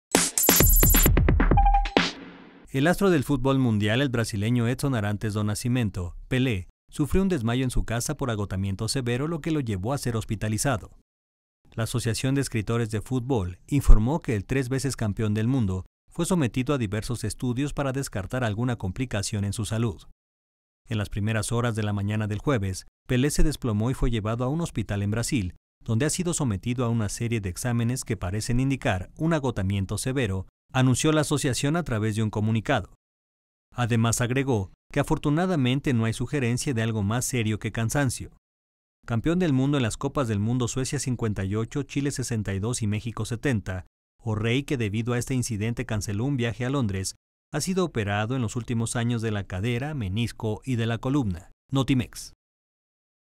“Pelé” sufrió un desmayo y fue ingresado a un hospital
El astro del futbol mundial, el brasileño Edson Arantes do Nascimento “Pelé”, sufrió un desmayo en su casa por “agotamiento severo” lo que lo llevó a ser hospitalizado.